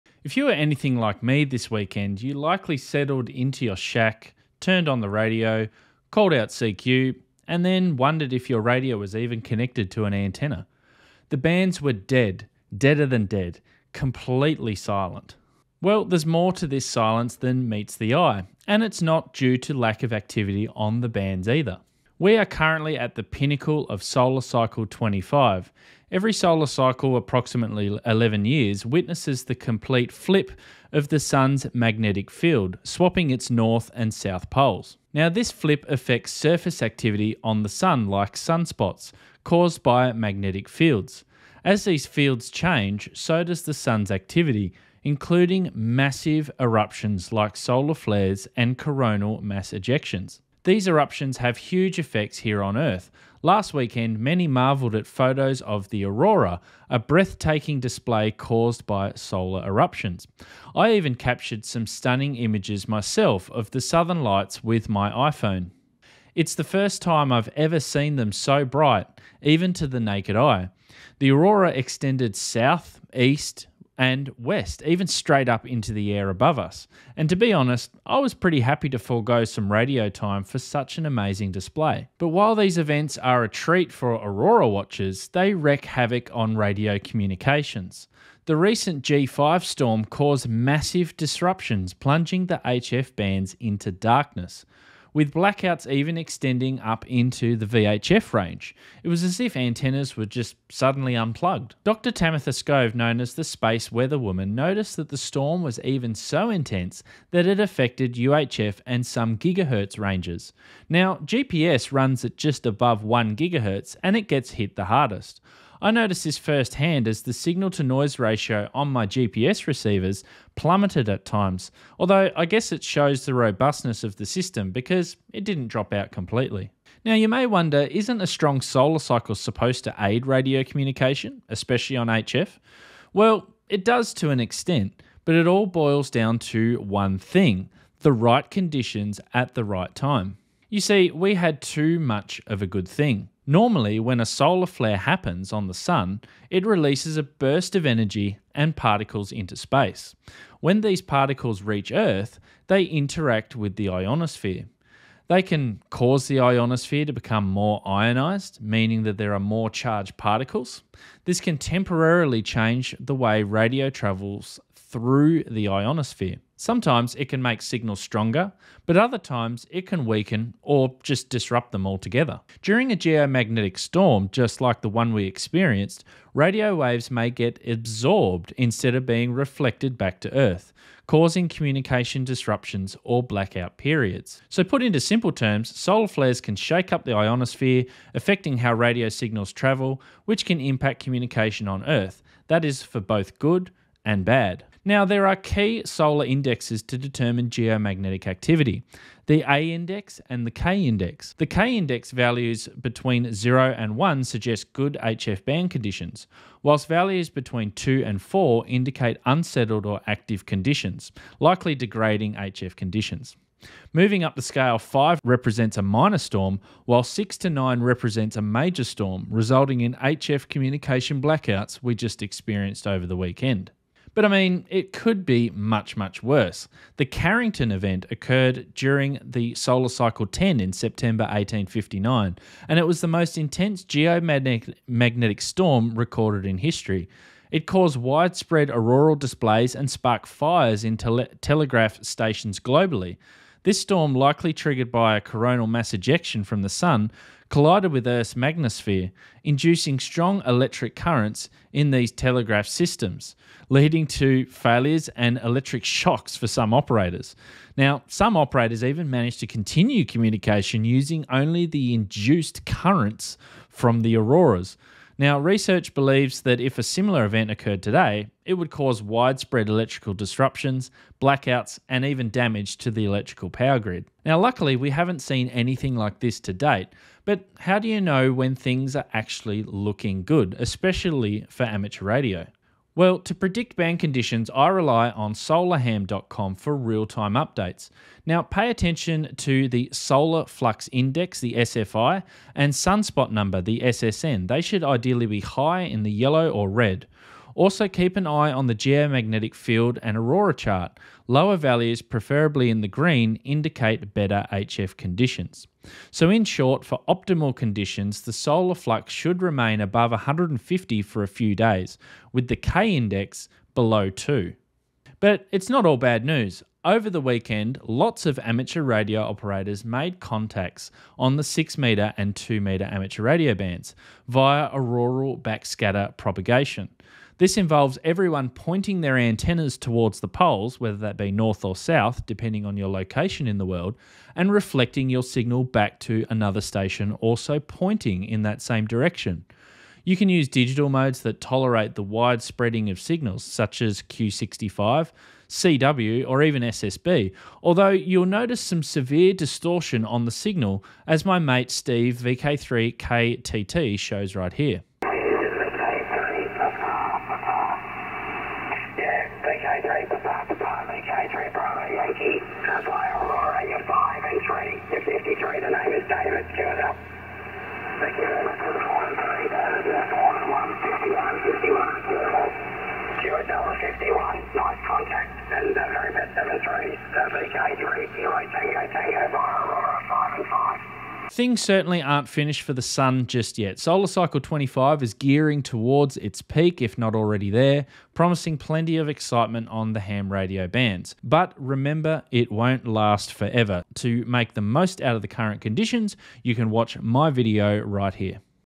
Unlocking the Mystery: Why the HF Bands Are DEAD
HF radio was completely silenced over the past weekend. We get to the bottom of what caused it, but the benefits it can provide to VHF ham radio operators.

➡️ Stuff I use in the shack
Outdoor GPS Antenna
On Air Sign
My Mic

⏱️Chapters⏱️
0:00 What has happened?
0:53 Aurora photos
1:22 Bad for Radio & GPS!!
2:06 Too much of a good thing
3:14 Key solar indexes you need to know
3:43 The Carrington Event
4:34 How to tell when HF is going to be good
5:15 It's not all bad news though..

🅰️ Amazon Store